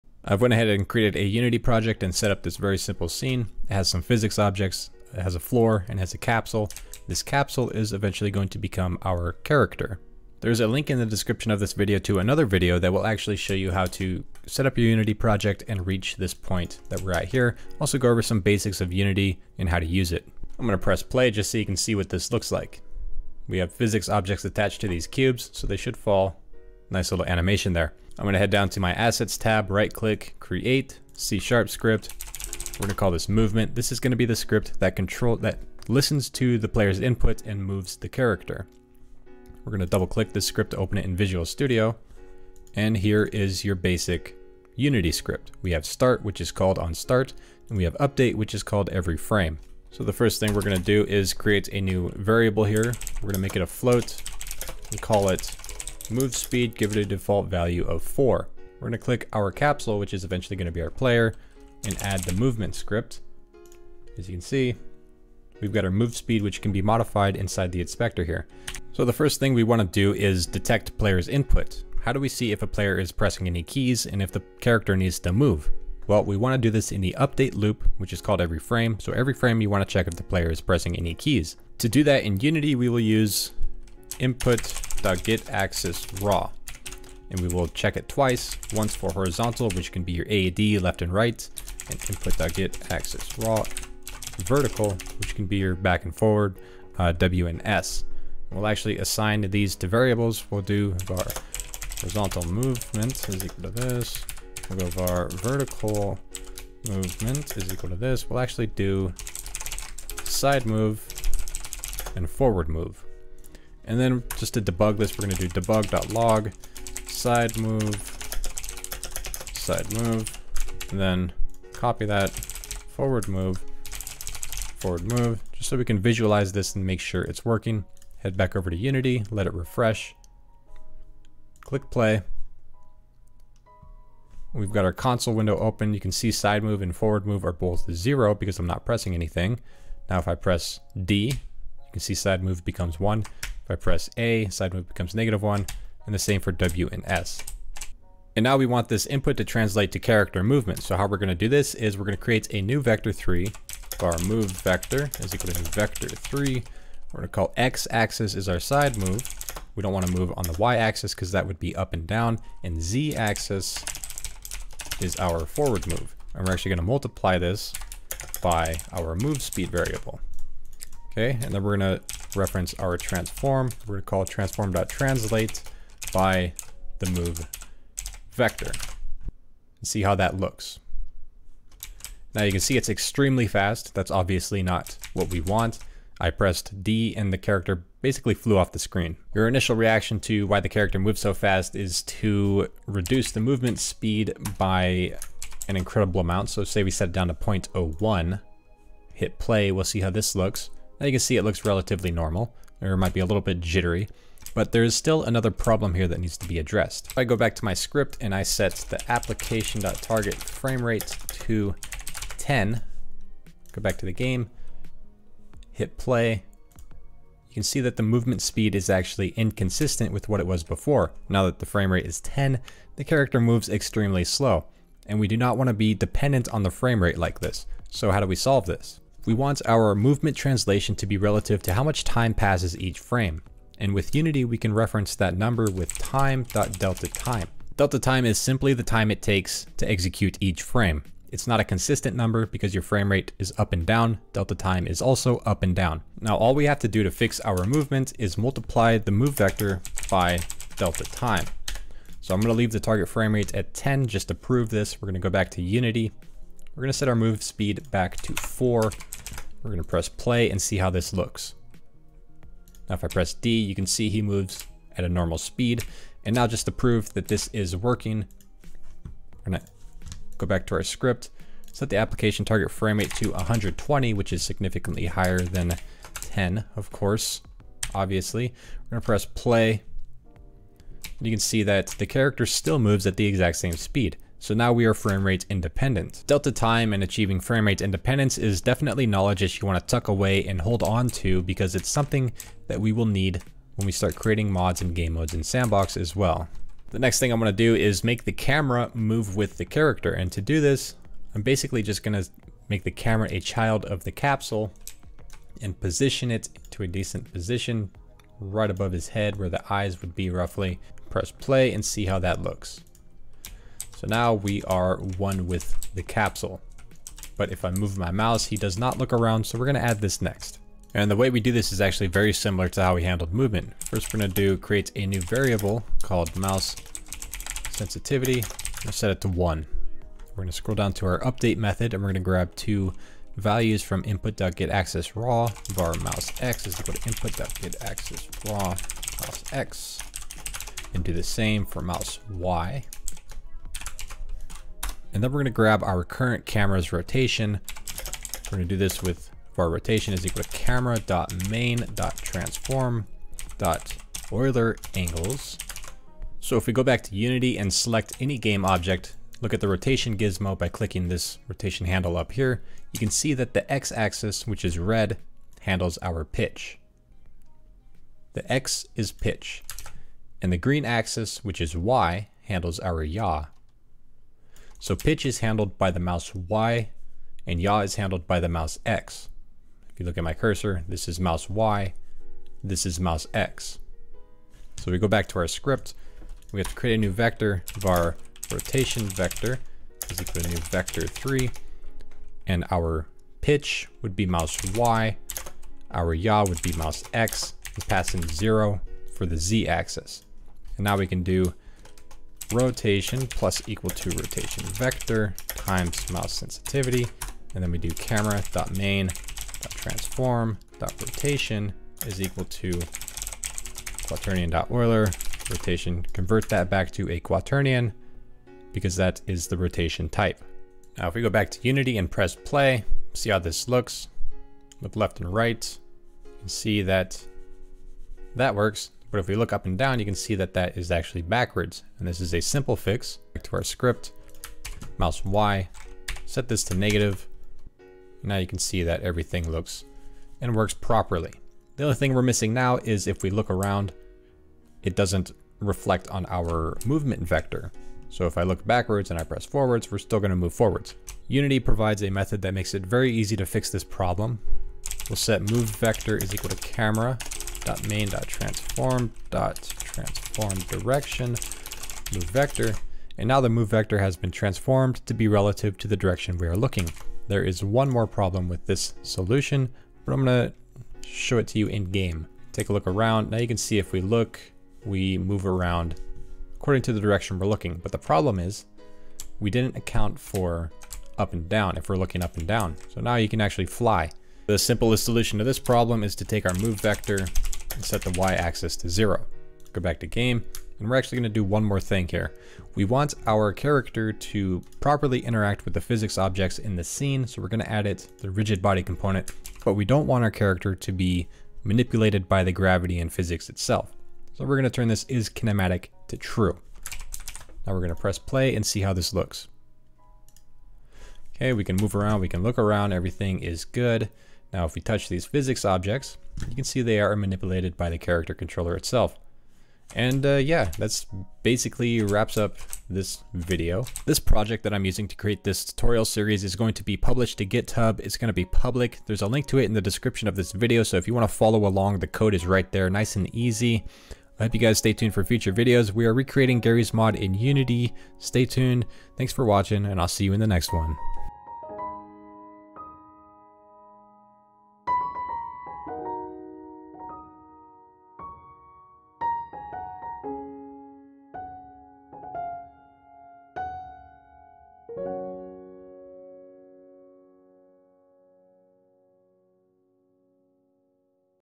Creating Gmod in Unity - Character Controller
This is a game development/c# series that will go on until the release of S&box.  By that time you will have learned many things that can be transferred to development of mods and gamemodes for S&box, and we will begin new tutorials that dig into S&box rather than Unity.

But for now, let's create Garry's Mod in Unity.